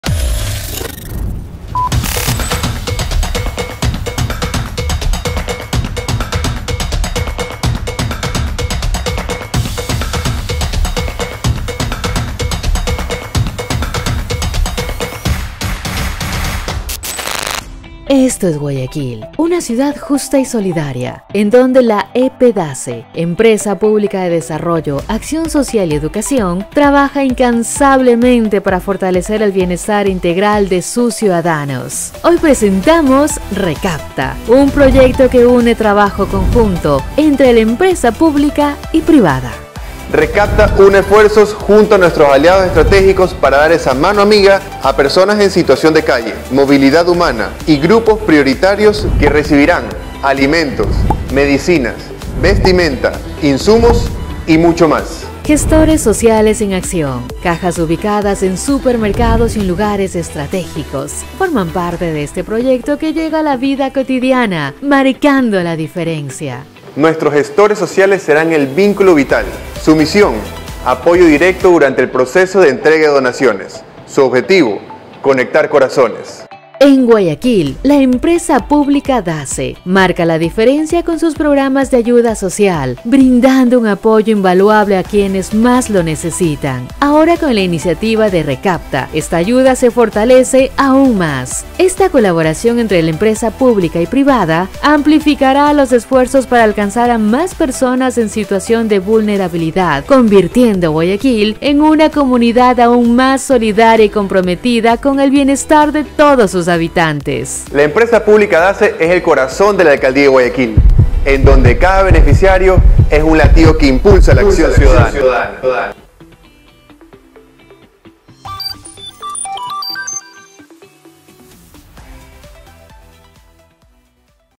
Proyecto Recapta 🤝
📣 Te presentamos nuestro proyecto Recapta, que tiene como objetivo unir esfuerzos entre la empresa pública y privada para ayudar a grupos vulnerables 🫱🏻‍🫲🏾.